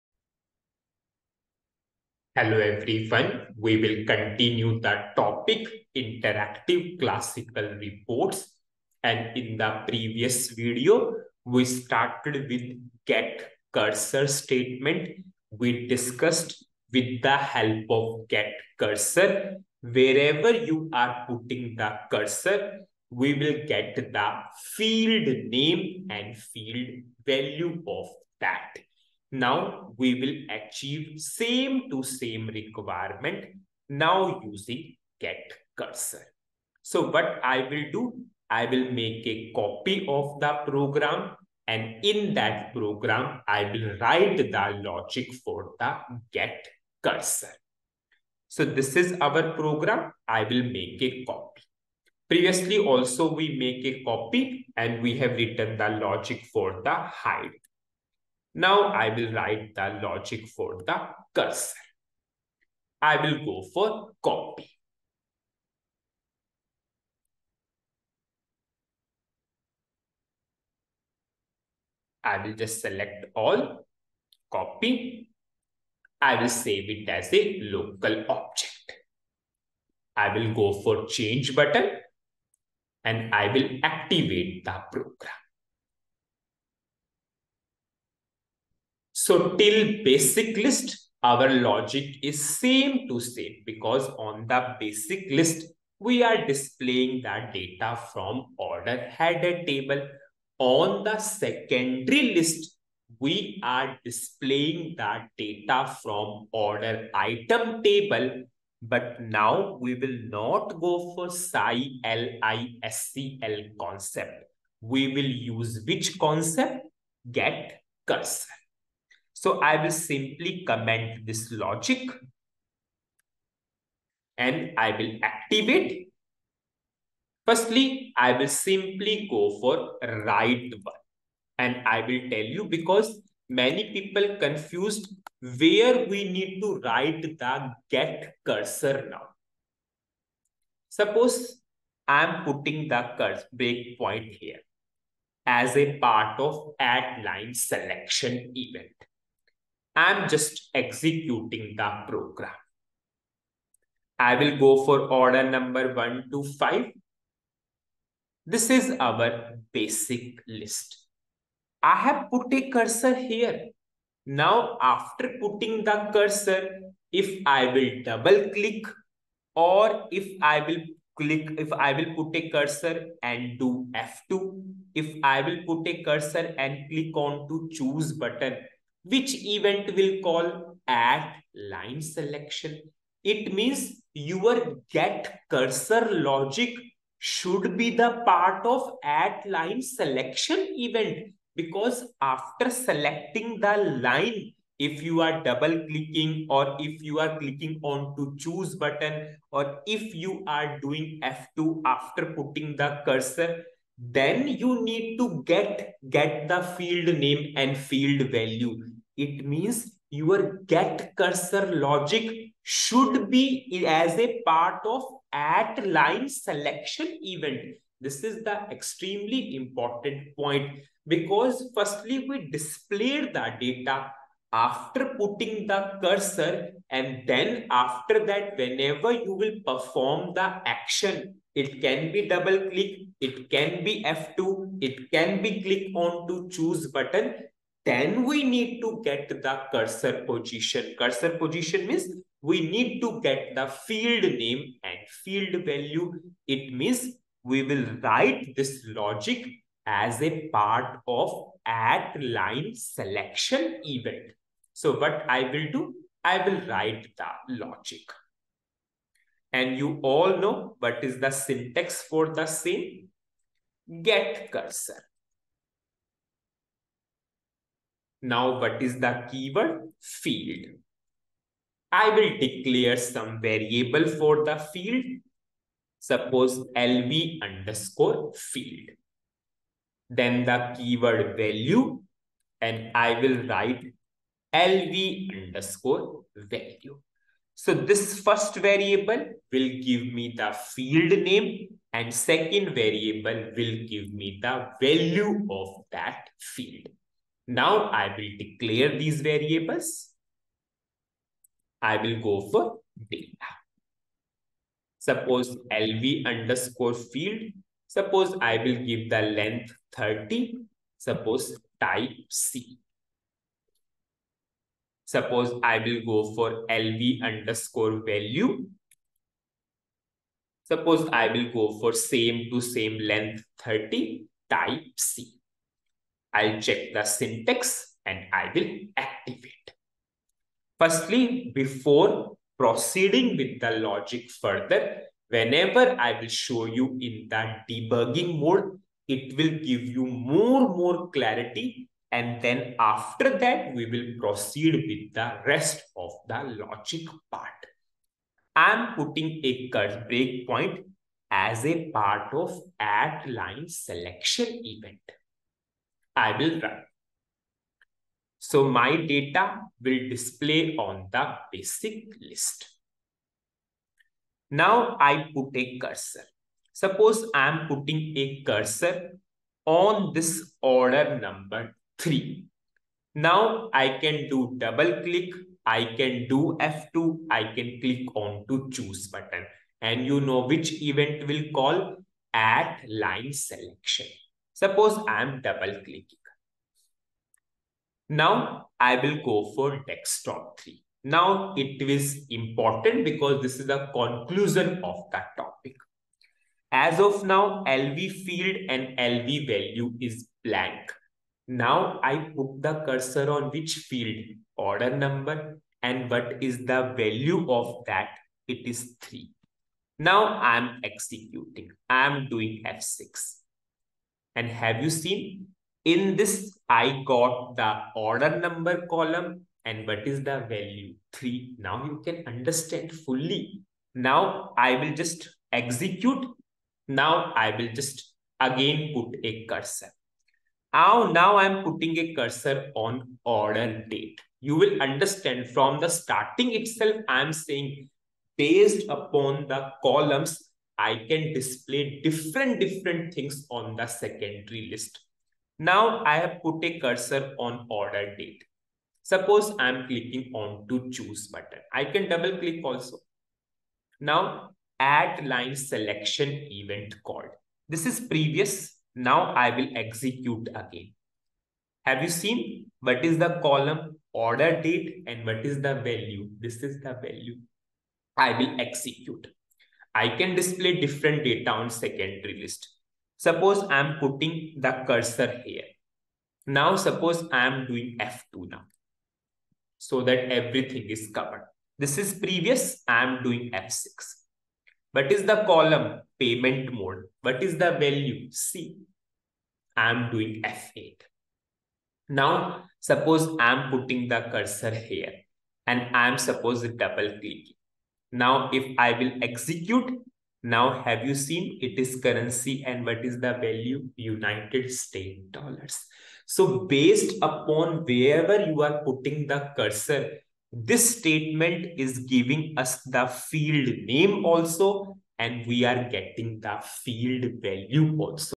84 - ABAP Programming - Interactive Classical Report Statements - GET CURSOR Part2
-Written the Syntax for GET CURSOR Statement as a Part of AT LINE-SELECTION Event.
-Understanding of Field Names and Field Values in the Debugging Mode, returned from GET CURSOR Statement.